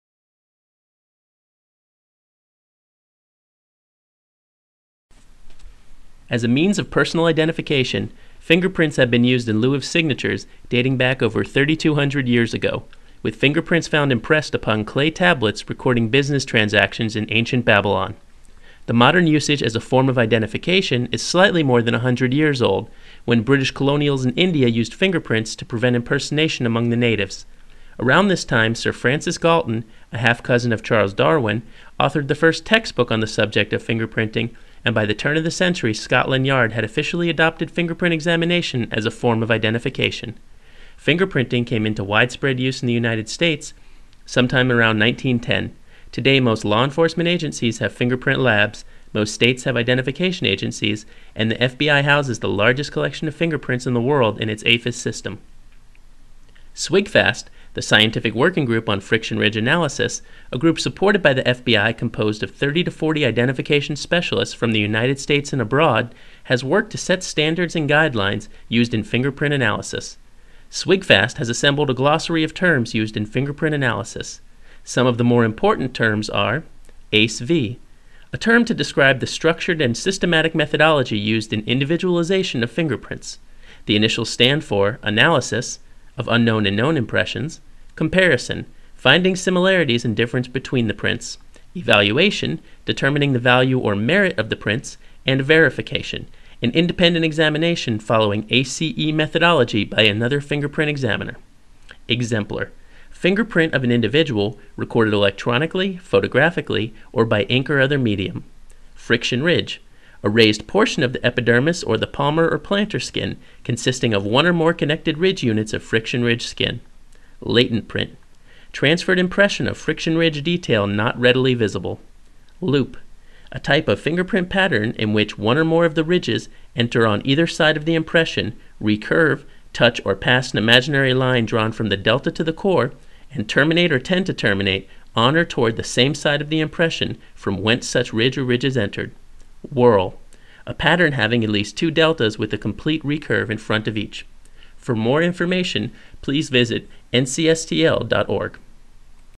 NCSTL Forensic Exclusive - History of Fingerprints
This audio/visual presentation discusses the history of fingerprints from ancient times to the 21st Century.